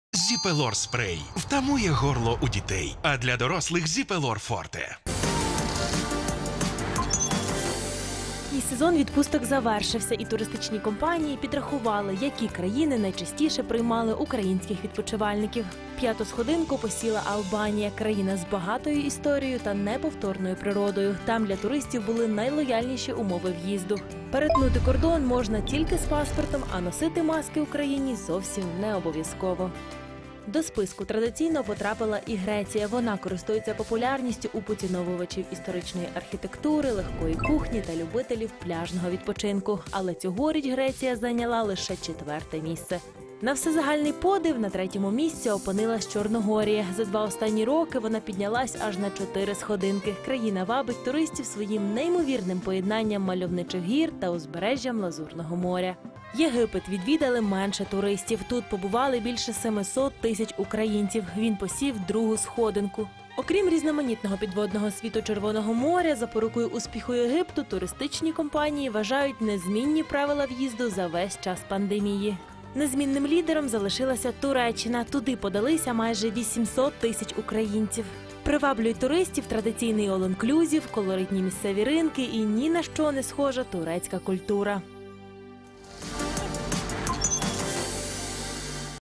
Які країни стали лідерами літнього відпочинку для українців
Літній сезон відпусток завершився. І туристичні компанії підрахували, які країни найчастіше приймали українських відпочивальників. Незмінним лідером залишилася Туреччина. Туди подалися майже 800 тисяч українців. Приваблює туристів традиційний "all inclusive", колоритні місцеві ринки і ні на що не схожа турецька культура.